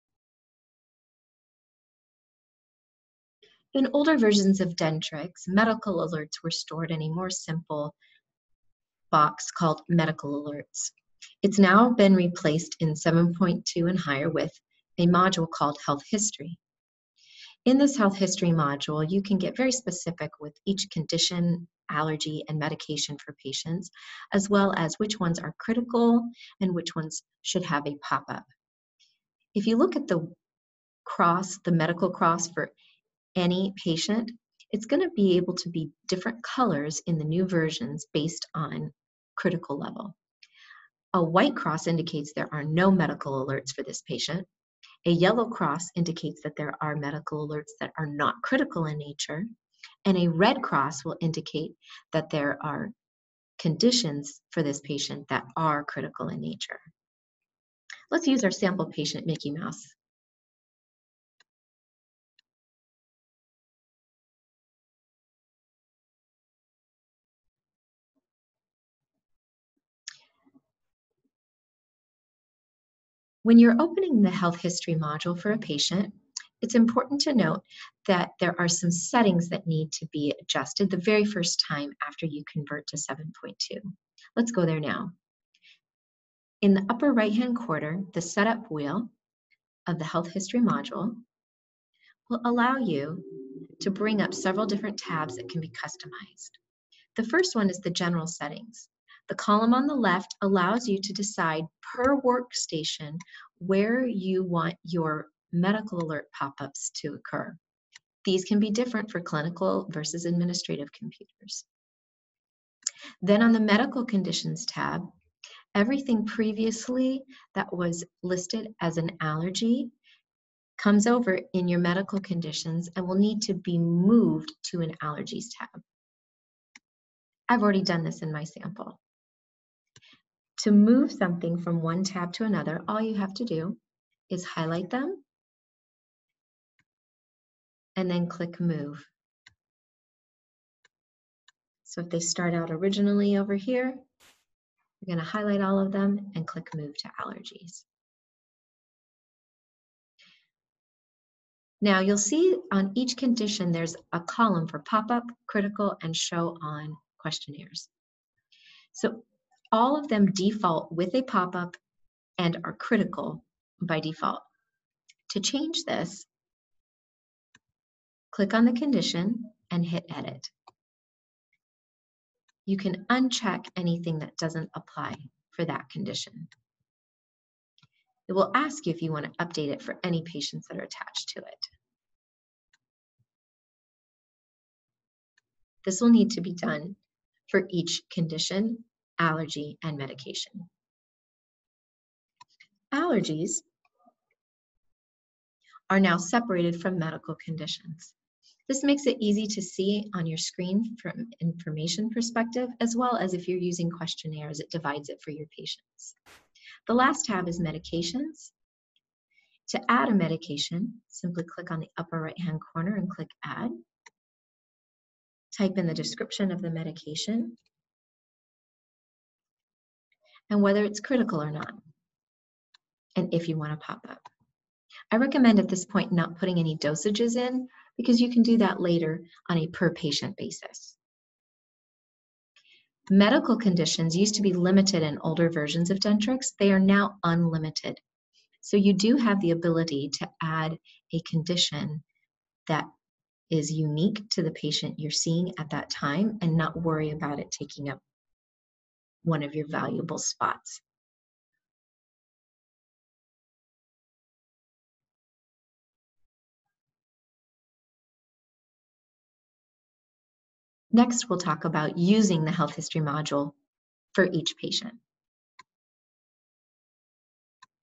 Dentrix Health History Setup G7.2 and Higher by Practice Strategies
In this short video from Practice Strategies, learn to how to set up/customize your Health History module in Dentrix versions G7.2 and higher.